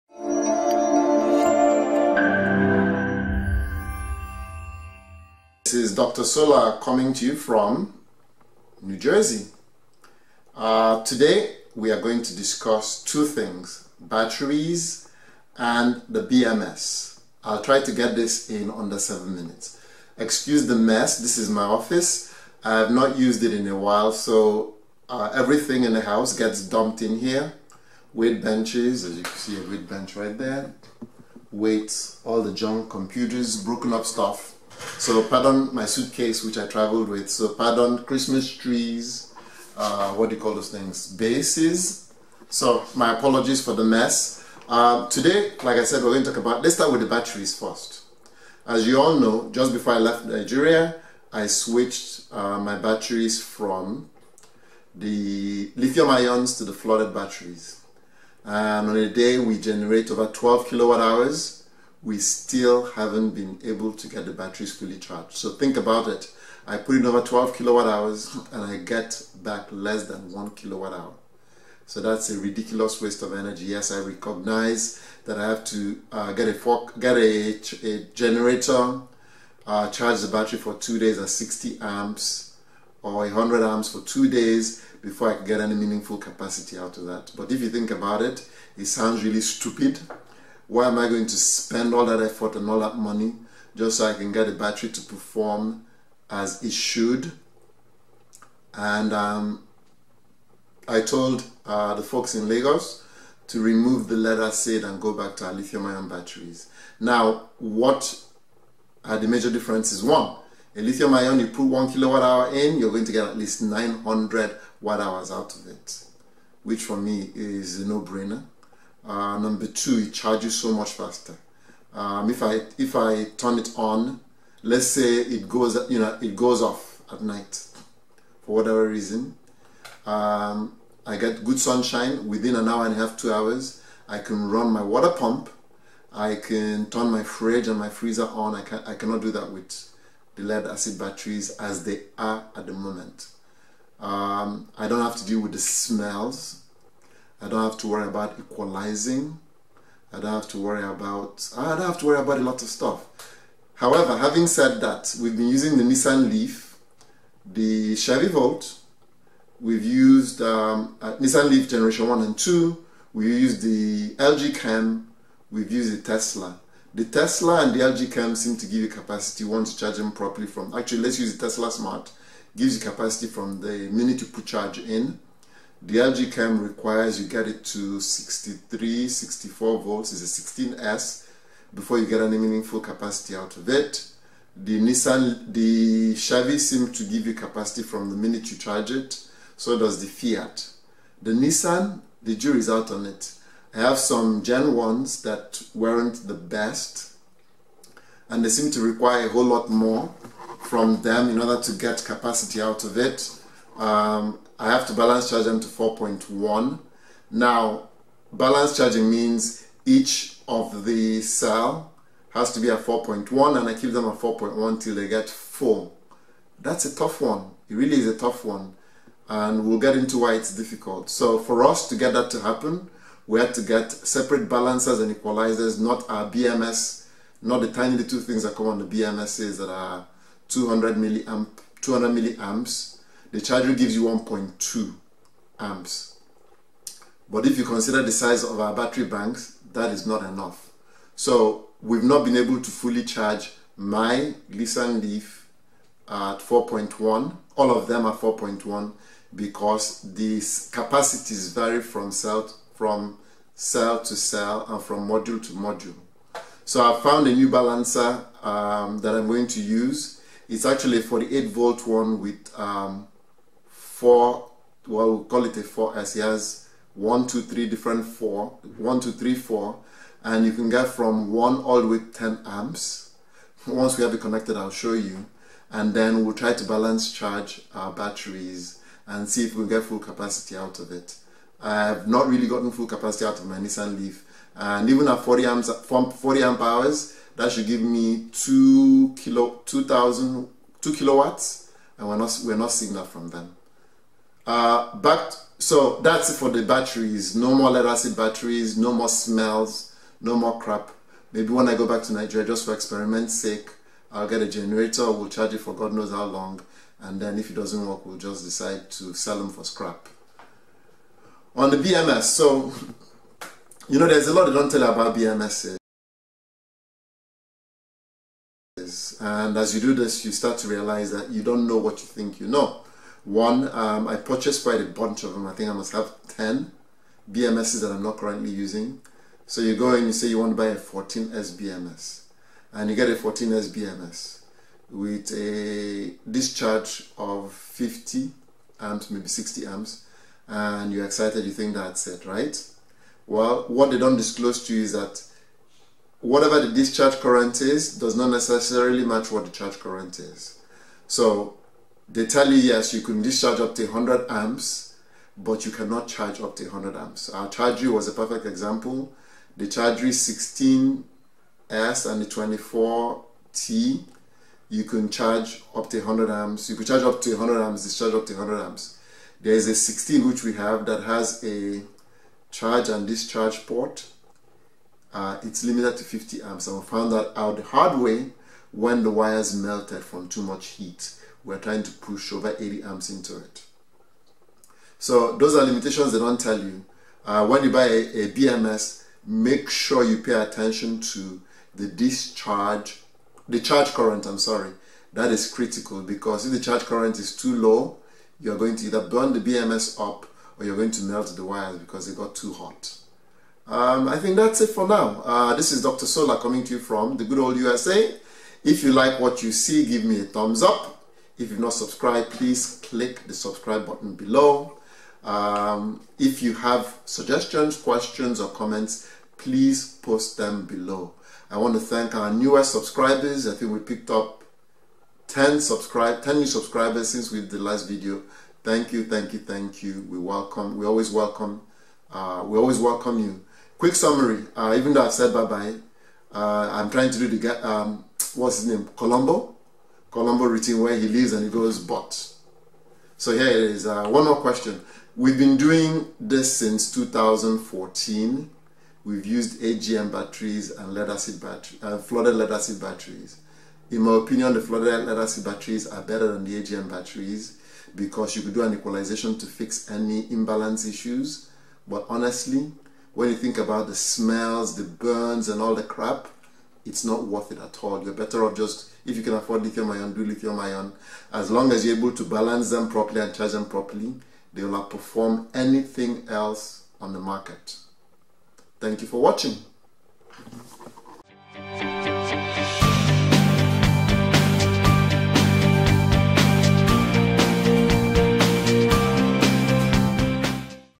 Lead acid vs Lithium Ion Batteries and what do you know about BMS'?
I switched to lithium ion batteries last year and 2 months ago I switched back to lead acid batteries. DUMB MOVE. I am hating it
You think you know about BMS's and then you realize you don't. Watch to find out my amazing AHA moment